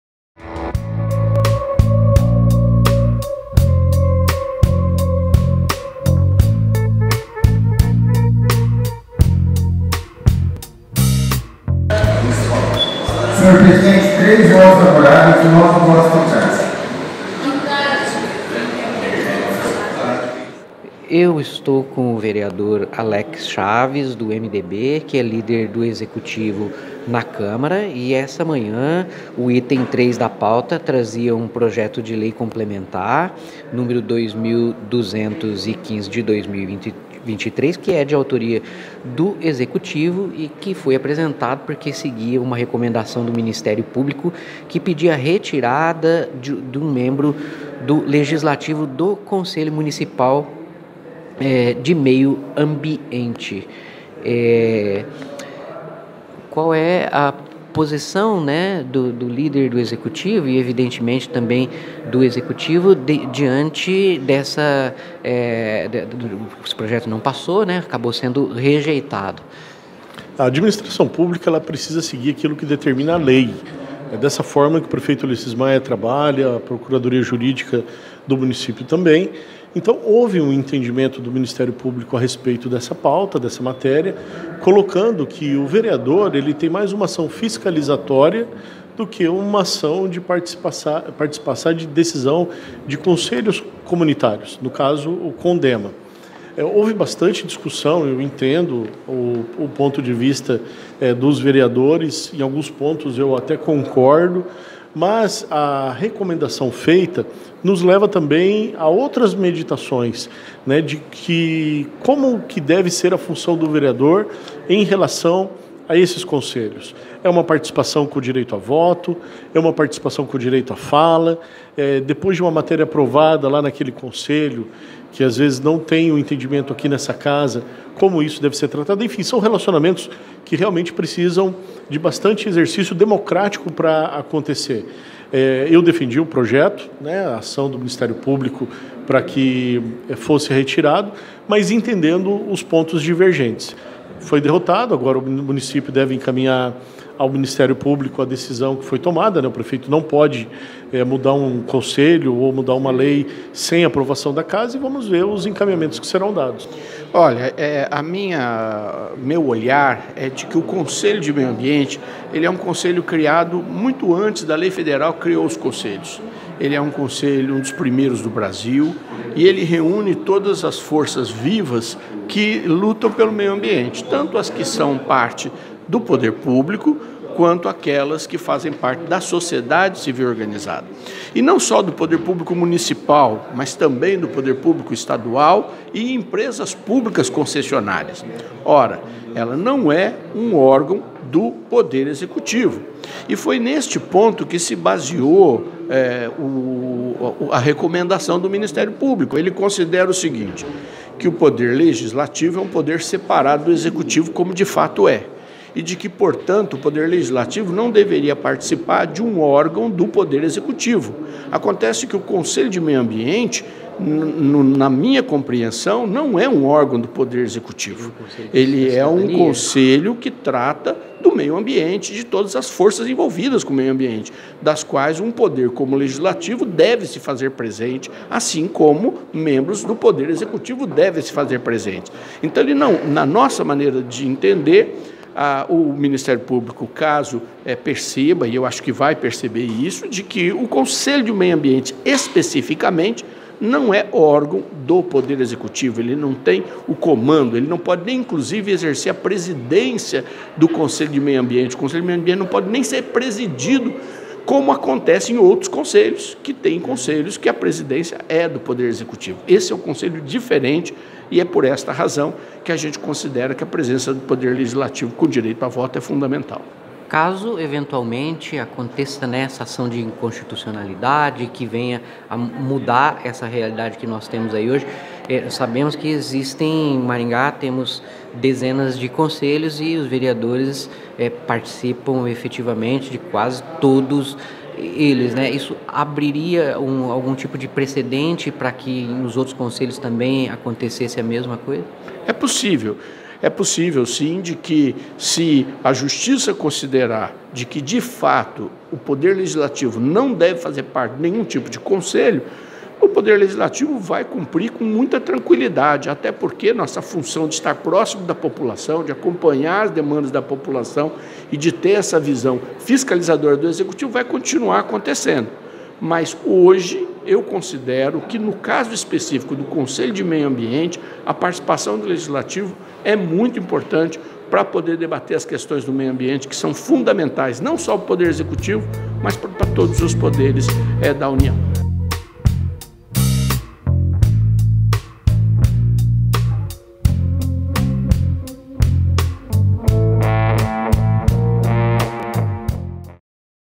Vereadores contrariam recomendação MP e rejeitam projeto que elimina voto do Legislativo no Comdema
O projeto do Executivo que foi enviado ao Legislativo depois que o Ministério Público recomendou a exclusão de membros do Legislativo do Conselho Municipal de Meio Ambiente - COMDEMA foi votado e rejeitado por 10 votos a 3 na manhã de hoje, 6, durante votação na sessão ordinária da Câmara de Maringá.

Se aprovado, o projeto alteraria a redação de dispositivos da Lei Complementar nº 1.093, de outubro de 2017 e o Legislativo passaria a ter apenas uma cadeira no COMDEMA, mas não teria mais direito a voto.

De acordo com a recomendação do MP, os Conselhos Municipais são orgãos vinculados ao Executivo e o artigo 7º da Constituição do Estado do Paraná preconiza a harmonia dos Poderes, bem por isso, o Legislativo não poderia ter assento no COMDEMA.  O pedido do MP é específico ao COMDEMA, mas a aprovação do projeto poderia ter como consequência a saída de representantes do Legislativo de todos os 54 Conselhos Municipais.

O vereador Alex Chaves (MDB) defendeu a aprovação do projeto na tribuna e na entrevista que concedeu a OFATOMARINGA.COM, e ressaltou que o MP já teria advertido que caso o projeto não passasse na Câmara, poderia propor uma Ação de Inconstitucionalidade.

Já o vereador Sidnei Telles (Avante) votou contra a aprovação e justificou dizendo "que o Comdema é um Conselho que foi criado antes da Lei Federal que criou os Conselhos". Para Telles, ao contrário do que sustenta o MP, os Conselhos não são orgãos do Poder Executivo, mas da Sociedade Civil Organizada. "O Conselho do Meio Ambiente é um orgão que trata das questões de defesa do Ambiente e por isso o Legislativo tem que se fazer presentes; espero que o MP perceba que há um equívoco na sua visão. Se a Justiça considerará que o Legislativo não pode fazer parte dos Conselhos, vamos cumprir com tranquilidade, mas esperamos que essa posição seja revisada pelo MP", disse Telles.